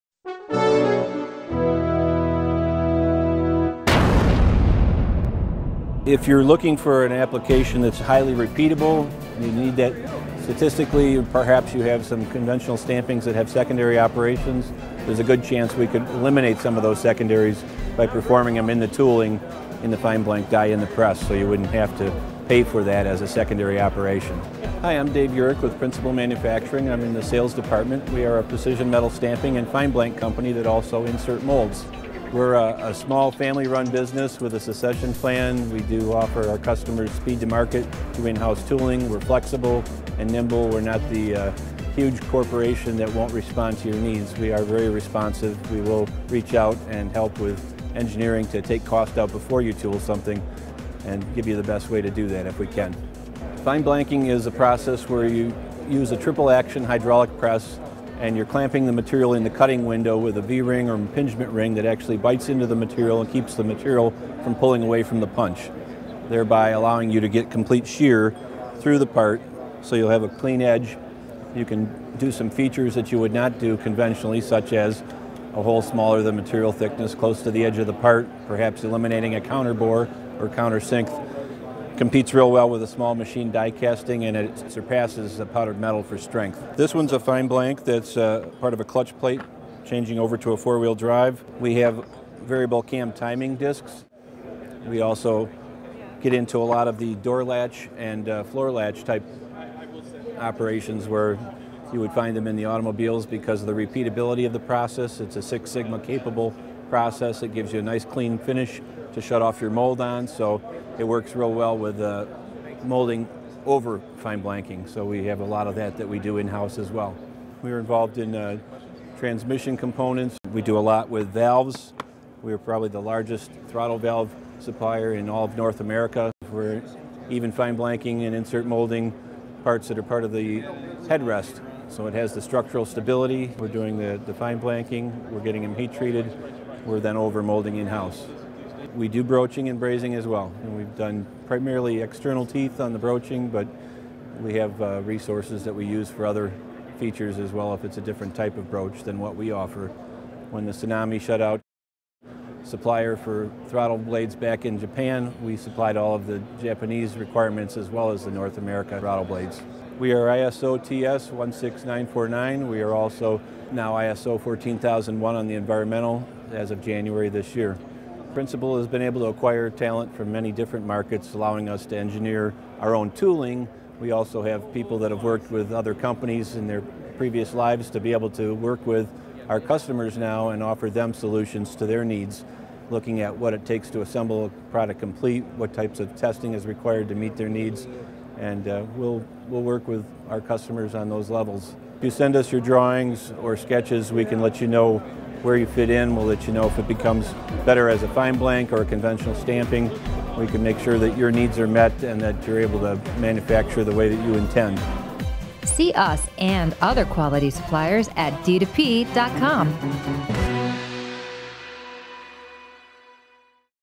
Engineered Fineblanking & Stampings | Principal Manufacturing | Broadview, IL
Principal Manufacturing Corporation is another preferred American supplier of contract manufacturing services you will find only at a Design-2-Part trade show

Principal Manufacturing Corporation is a TS 16949 and ISO 14001 certified company offering engineered solutions for automotive, industrial and OEM companies worldwide. They are a full service supplier for ready to use parts, offering engineering assistance to reduce cost and speed to market through their vast in-house tooling.

Their core capabilities include fineblanking up to 630 tons, conventional stamping up to 600 tons, and plastic injection molding. Their secondary operations include: automated assembly, brazing, broaching, CNC machining, counter boring, deburring, drilling, grinding, milling, orbital riveting, staking, tapping, turning and vibratory tumbling.

Principal has been operating from its current facility in Broadview, IL since 1996.